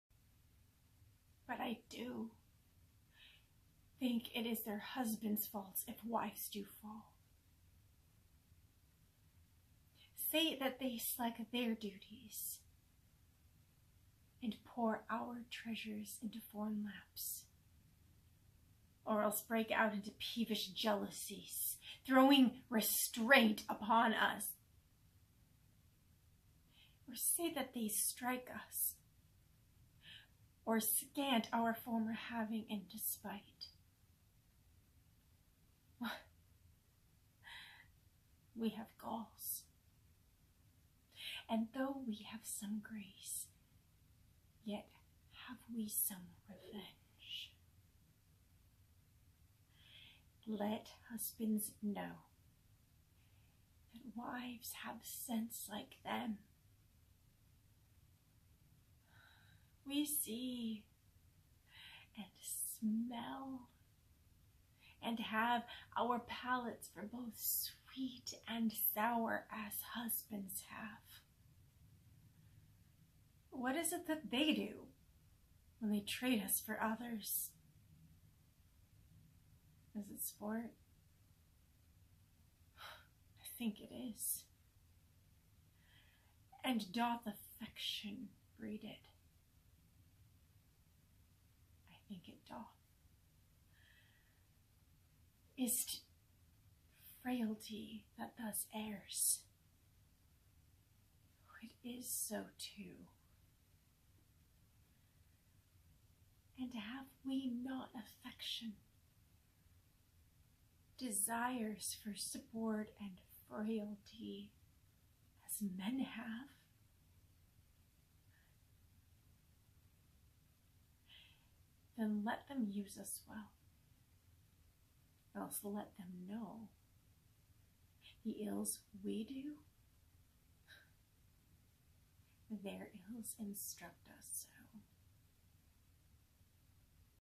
Othello, Act IV, Scene III (Emilia's Monologue)
Midnight Monologue Challenge
On behalf of The Playhouse in Las Vegas, NV, I’m participating in a new challenge. That is, to perform a favorite monologue (comedy or drama, classic or contemporary) completely removed from its thematic elements. No space, no time, no context, NO RESTRICTIONS!

I’m performing one of my favorite Shakespearean monologues: Othello, Act IV, Scene III, commonly known as "Emilia's Monologue." It's pretty overdone, honestly, but it's clear to anyone who knows me why it's one of my faves (while everyone is fawning all over Desdemona, here's Emilia over in Act IV, being all feminist and progressive).